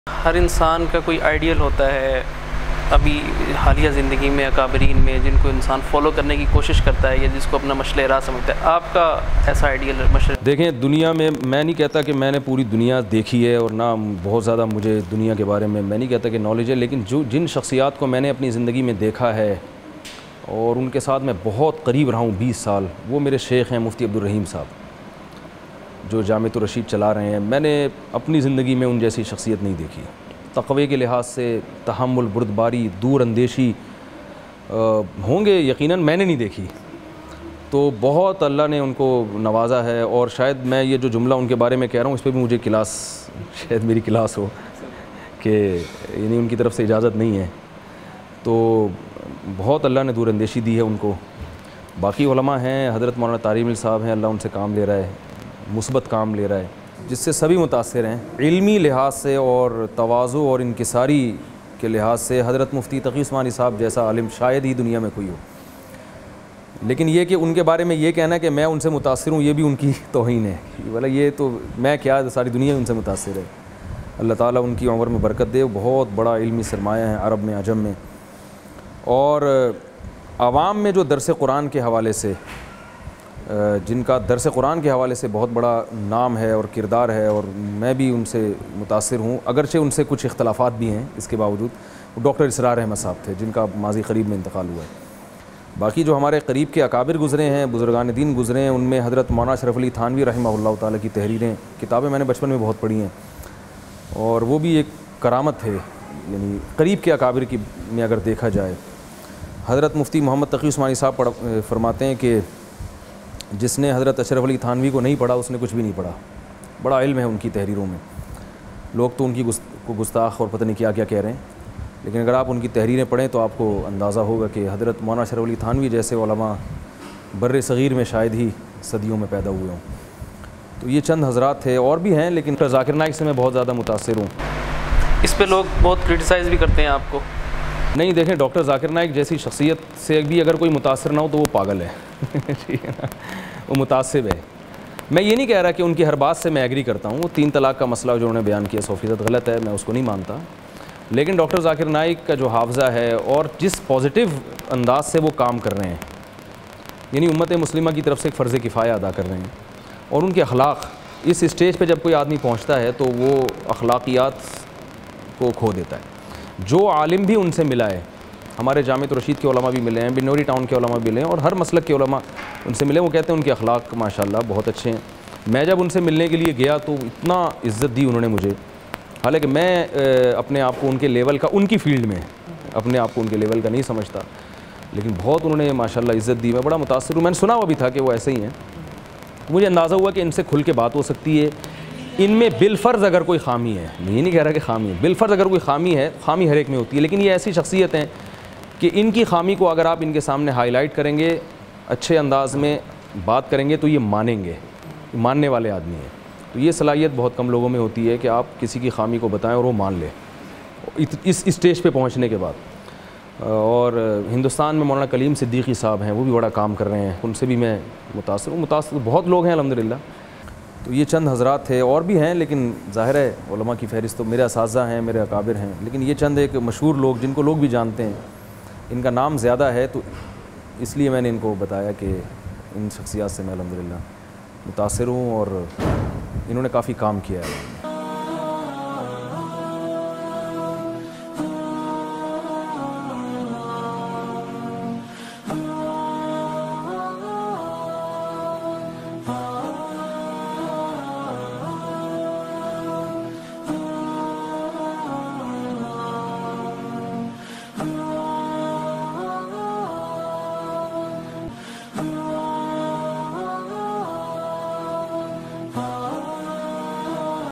My Favorite Personalities Mufti Tariq Masood Talk - مفتی طارق کی پسندیدہ شخصیات
مفتی طارق مسعود صاحب کی پسندیدہ شخصیات کون کونسی ہیں ؟ جانیے مفتی طارق مسعود صاحب کی زبانی